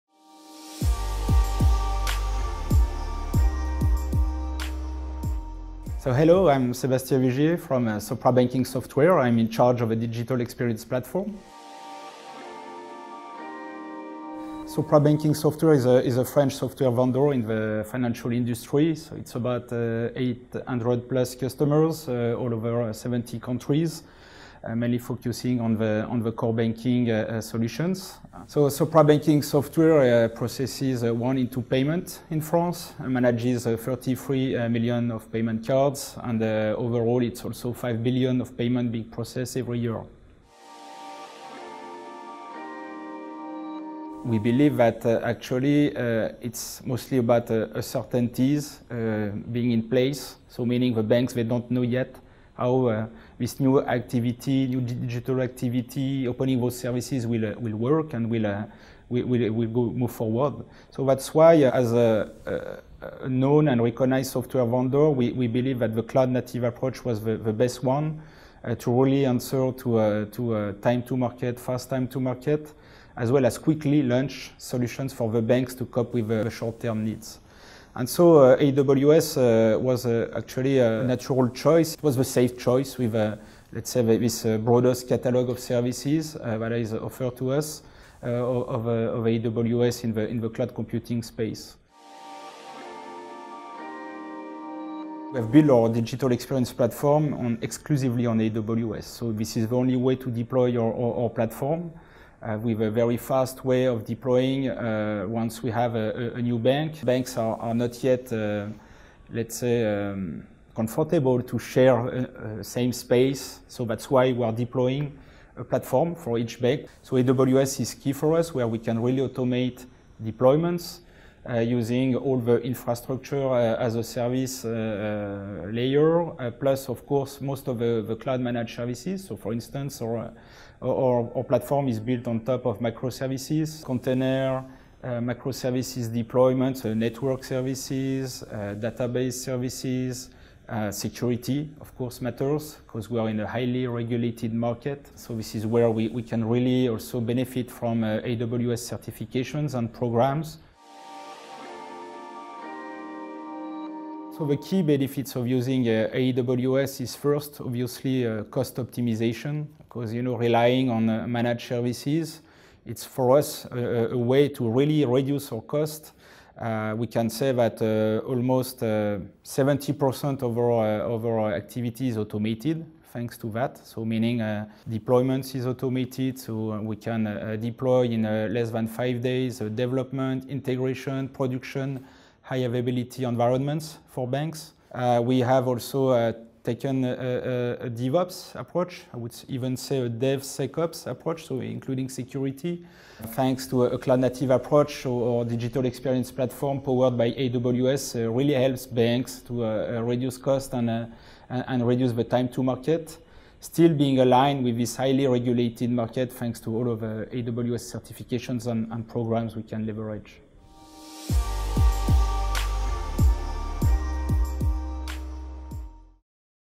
AWS & Sopra Banking | Sopra Banking automatise ses déploiements avec AWS
Sopra Banking utilise AWS car cela leur permet d'automatiser les déploiements. Ils optimisent leurs coûts et ont pu automatiser 70% de leur activité. AWS certifications pour un marché hautement réglementé. Retrouvez d'autres témoignages de clients AWS